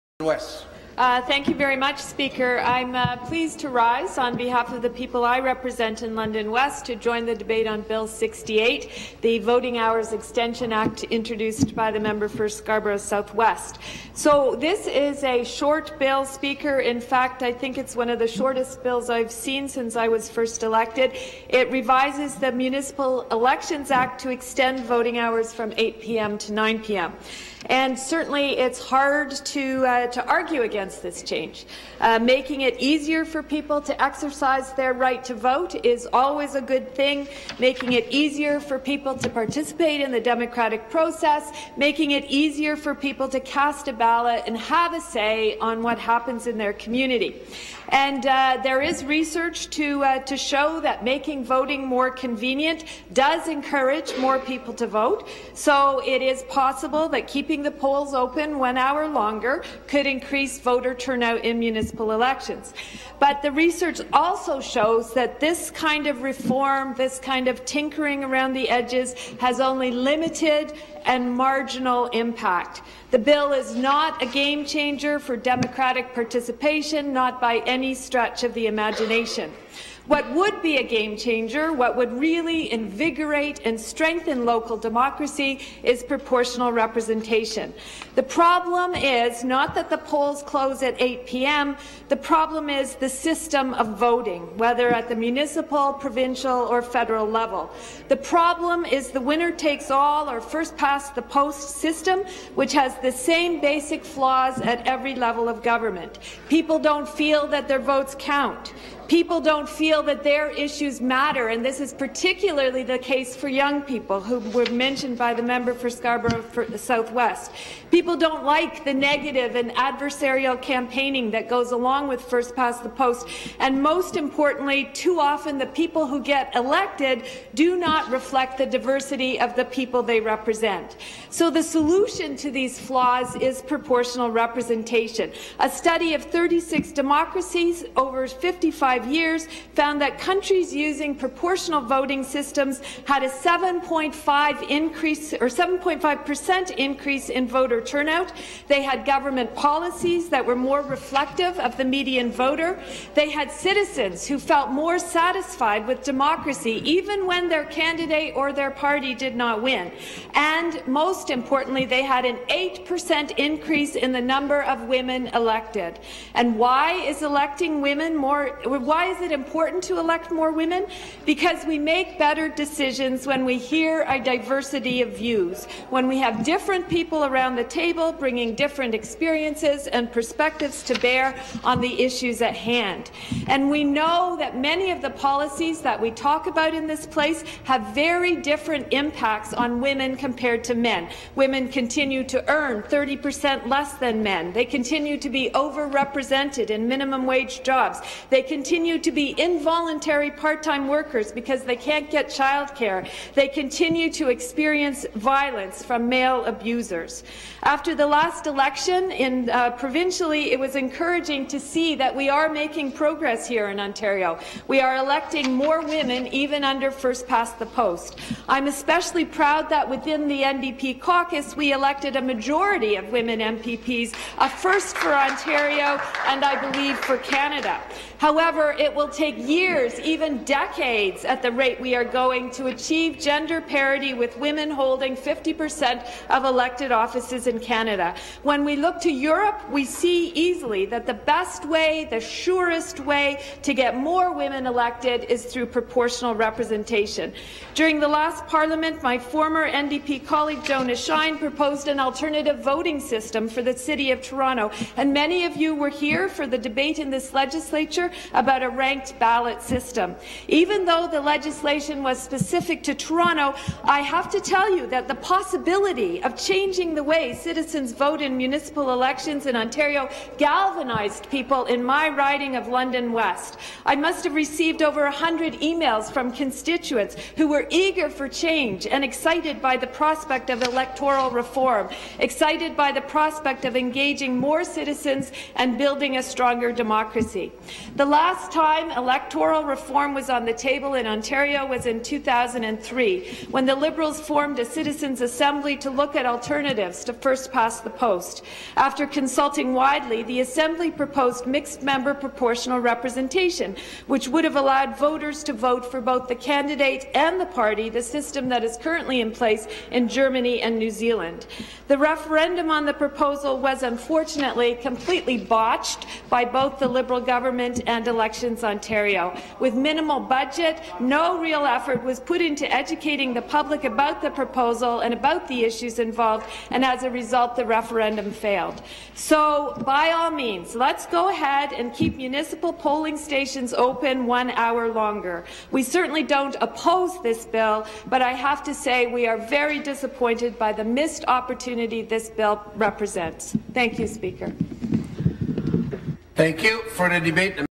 MPP Sattler's Remarks on the Voting Hours Extension Act
Ms. Peggy Sattler: I’m pleased to rise on behalf of the people I represent in London West to join the debate on Bill 68, the Voting Hours Extension Act, introduced by the member for Scarborough Southwest.

This is a short bill. In fact, I think it’s one of the shortest ones I’ve seen since I was first elected. It revises the Municipal Elections Act to extend voting hours from 8 p.m. to 9 p.m. Certainly, it’s hard to argue against this change. Making it easier for people to exercise their right to vote is always a good thing—making it easier for people to participate in the democratic process, making it easier for people to cast a ballot and have a say on what happens in their community. There is research to show that making voting more convenient does encourage more people to vote. So it is possible that keeping the polls open one hour longer could increase voter turnout in municipal elections. The research also shows that this kind of reform, this kind of tinkering around the edges, has only limited and marginal impact. The bill is not a game-changer for democratic participation, not by any stretch of the imagination.
What would be a game-changer, what would really invigorate and strengthen local democracy is proportional representation? The problem is not that the polls close at 8 p.m.; the problem is the system of voting, whether at the municipal, provincial or federal level. The problem is the winner-takes-all, or first-past-the-post, system, which has the same basic flaws at every level of government. People don’t feel that their votes count. People don’t feel that their issues matter, and this is particularly the case for young people, who were mentioned by the member for Scarborough Southwest. People don’t like the negative and adversarial campaigning that goes along with first-past-the-post. Most importantly, too often, the people who get elected do not reflect the diversity of the people they represent. The solution to these flaws is proportional representation.
A study of 36 democracies over 55 years found that countries using proportional voting systems had a 7.5% increase in voter turnout. They had government policies that were more reflective of the median voter. They had citizens who felt more satisfied with democracy even when their candidate or their party did not win.

Most importantly, they had an 8% increase in the number of women elected. Why is it important to elect more women? Because we make better decisions when we hear a diversity of views, when we have different people around the table bringing different experiences and perspectives to bear on the issues at hand. We know that many of the policies that we talk about in this place have very different impacts on women compared to men.
Women continue to earn 30% less than men. They continue to be overrepresented in minimum wage jobs. They continue to be involuntary part-time workers because they can’t get child care. They continue to experience violence from male abusers.
After the last election provincially, it was encouraging to see that we are making progress here in Ontario. We are electing more women, even under first-past-the-post. I’m especially proud that within the NDP caucus, we elected a majority of women MPPs, a first for Ontario and, I believe, for Canada. However, it will take years, even decades at the rate we are going, to achieve gender parity, with women holding 50% of elected offices in Canada. When we look to Europe, we see easily that the best way, the surest way to get more women elected is through proportional representation.
During the last Parliament, my former NDP colleague Jonah Schein proposed an alternative voting system for the city of Toronto. Many of you were here for the debate in this Legislature about a ranked ballot system. Even though the legislation was specific to Toronto, I have to tell you that the possibility of changing the way citizens vote in municipal elections in Ontario galvanized people in my riding of London West. I must have received over 100 emails from constituents who were eager for change and excited by the prospect of electoral reform, excited by the prospect of engaging more citizens and building a stronger democracy.
The last time electoral reform was on the table in Ontario was in 2003, when the Liberals formed a citizens’ assembly to look at alternatives to first-past-the-post. After consulting widely, the assembly proposed mixed member proportional representation, which would have allowed voters to vote for both the candidate and the party, the system that is currently in place in Germany and New Zealand.

The referendum on the proposal was, unfortunately, completely botched by both the Liberal government and Elections Ontario.